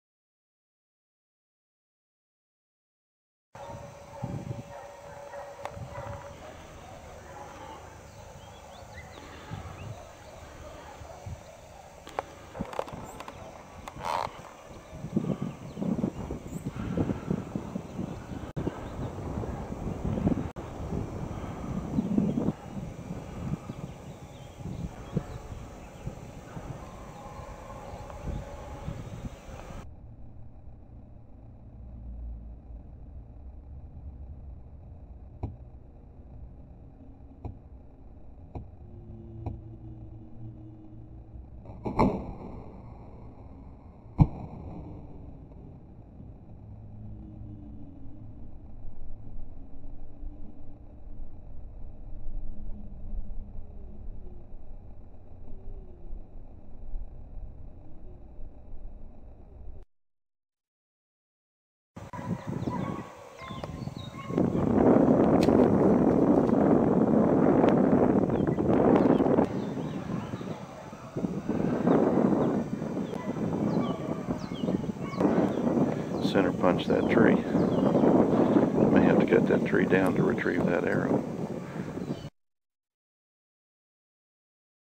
2006 Hog Hunt
My buddy Rick invited me to go chase down some hogs.  He was behind the camera.  I was behind the bow.  The hogs had a good laugh.